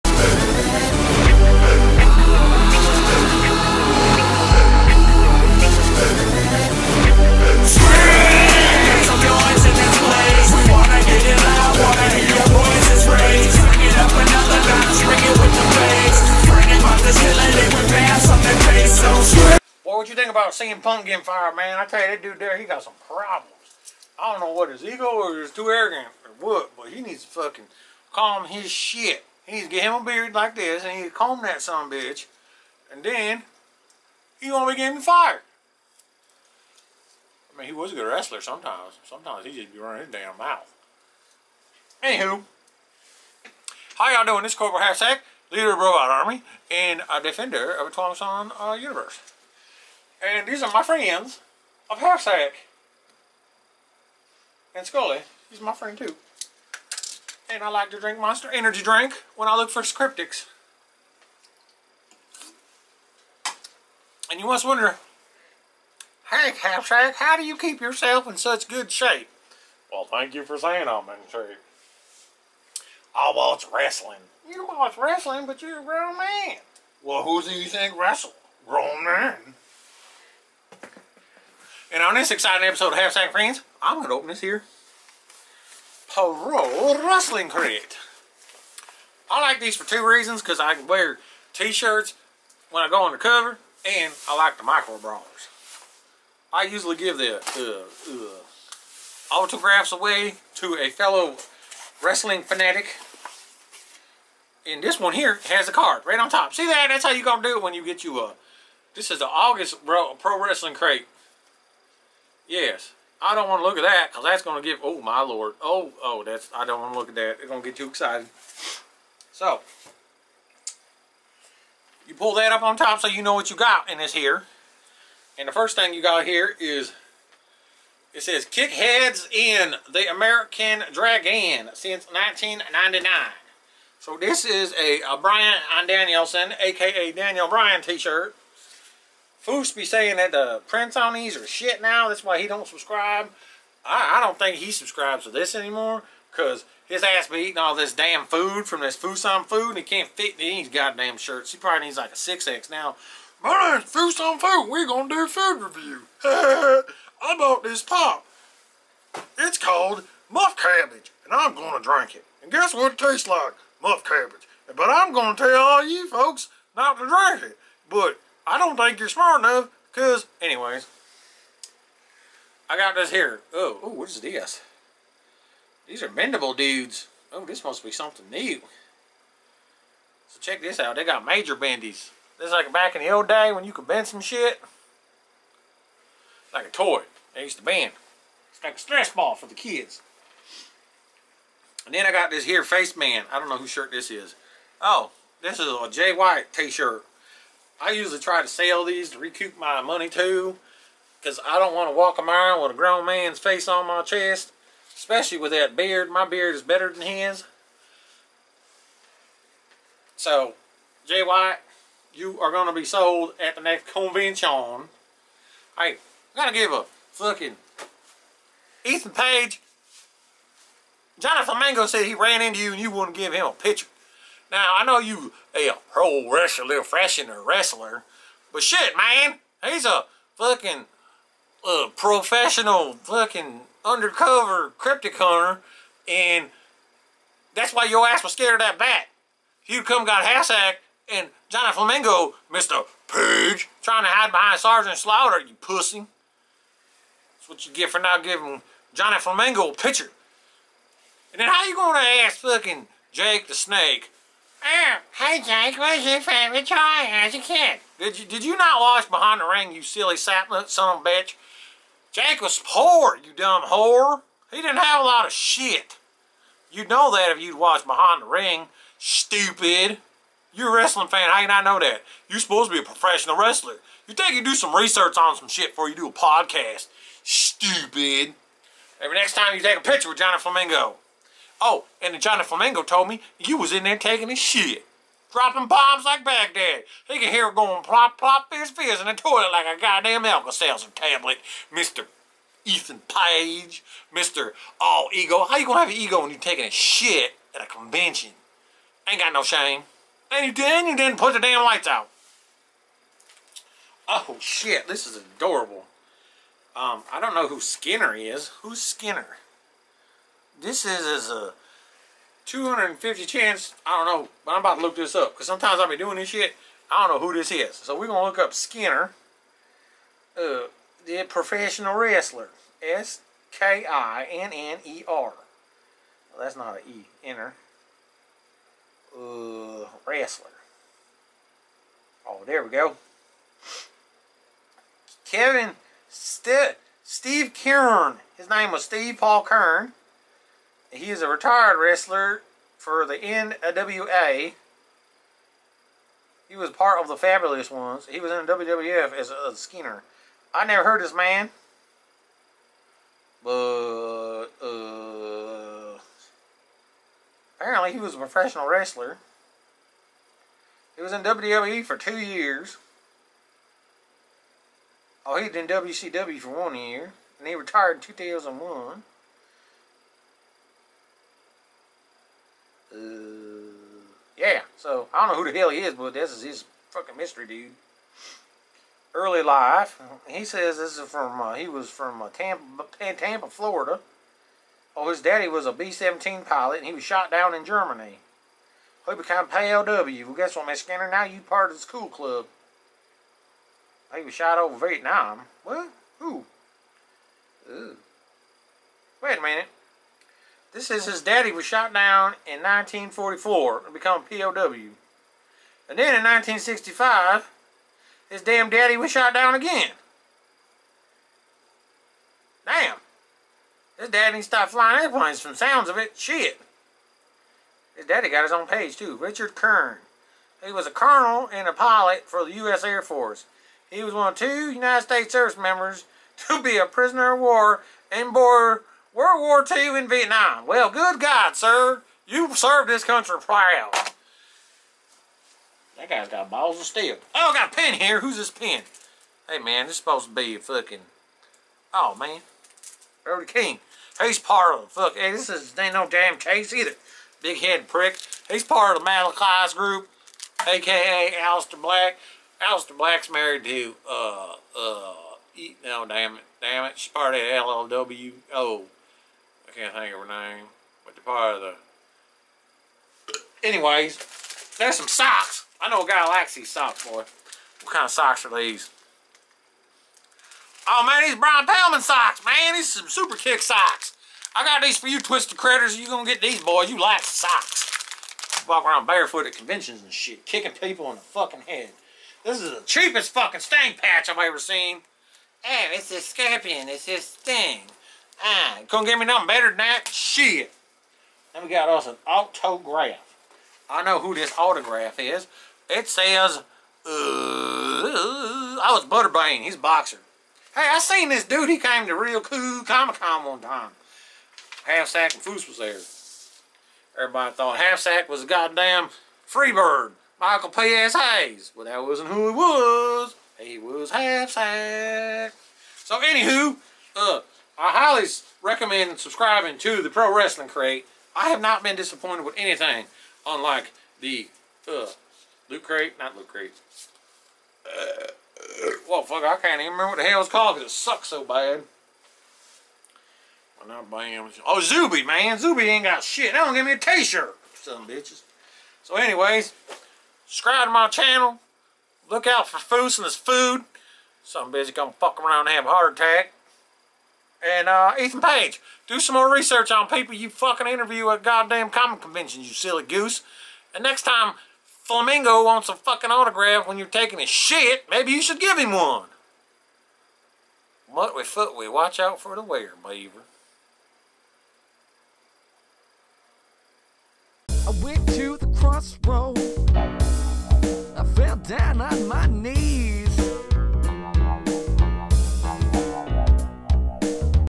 Pro Wrestling Crate August 2023 Unboxing and Review
Join Half-Sank and friends as they unbox yet another EXCITING Pro Wresting Crate and give a SHOUT OUT to All Ego Ethan Page!